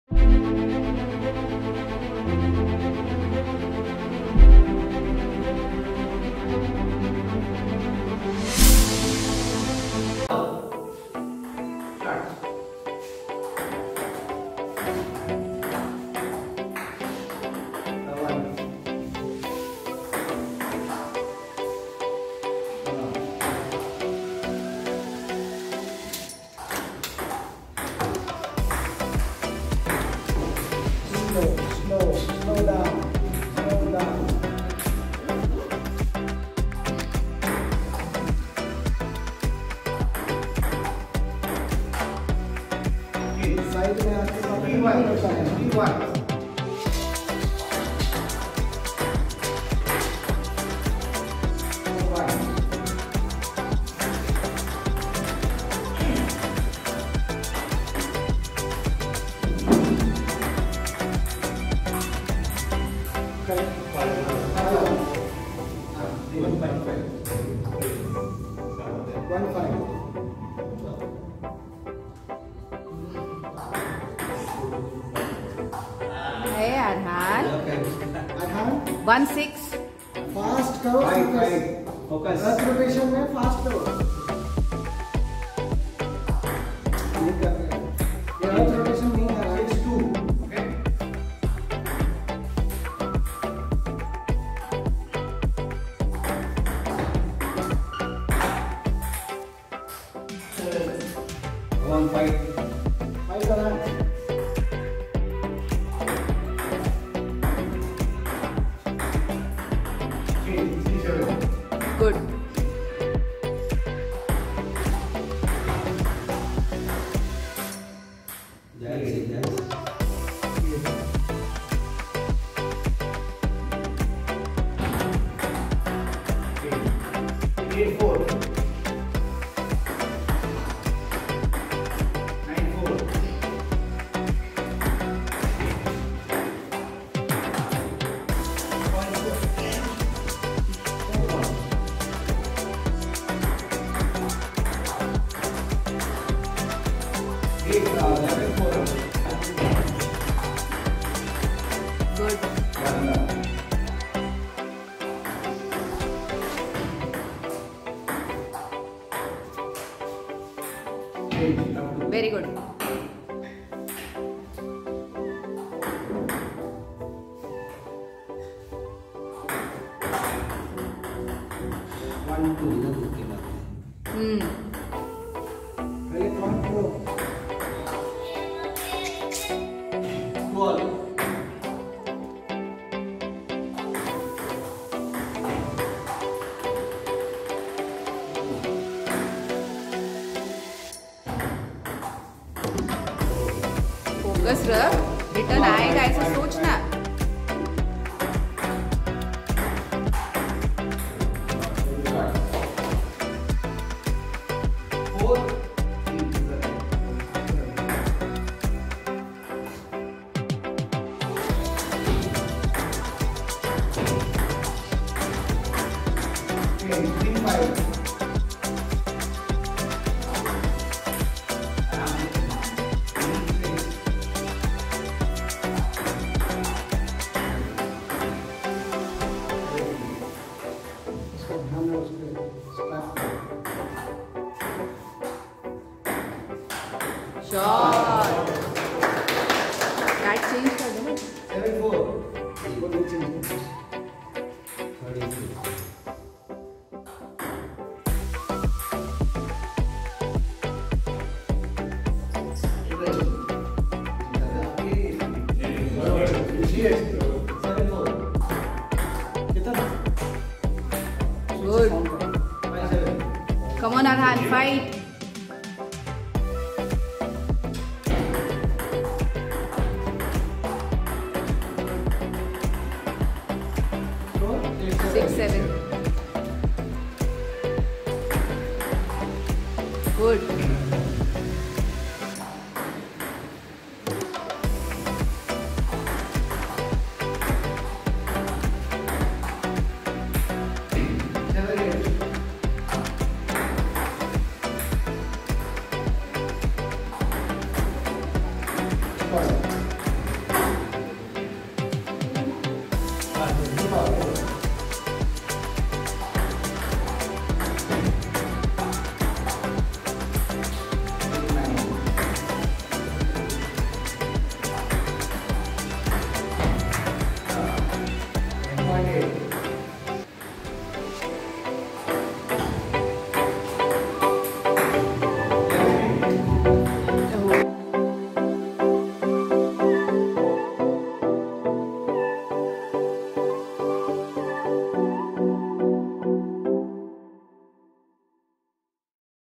Table Tennis: Arhaan vs Shaurya (Finals)
Table Tennis Premier League (2023-24) held at Palladium Homes on 5th August 2023 (kids category)